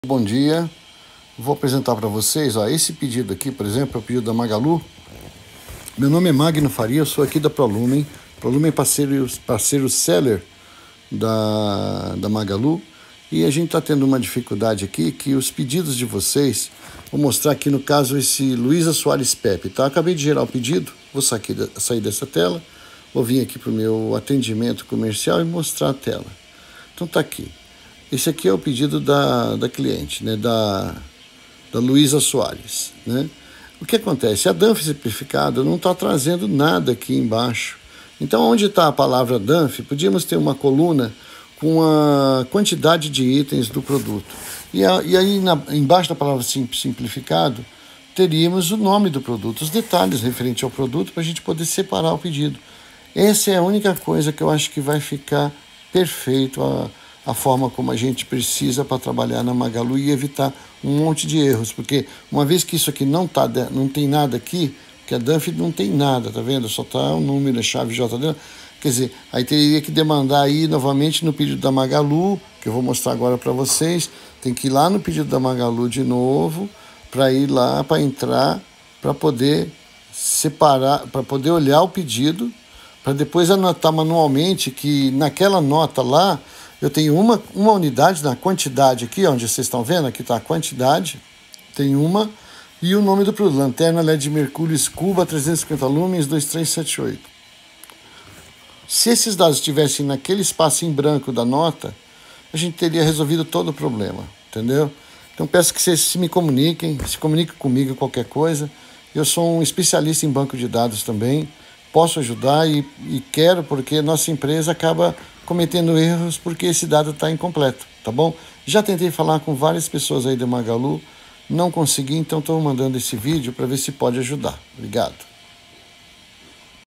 Magalu ajustes plataforma
Vídeo ilustrativo sobre ajustes necessários na plataforma para aumentar a usabilidade do sistema Magalu para o Seller .
Realizado por Magno Faria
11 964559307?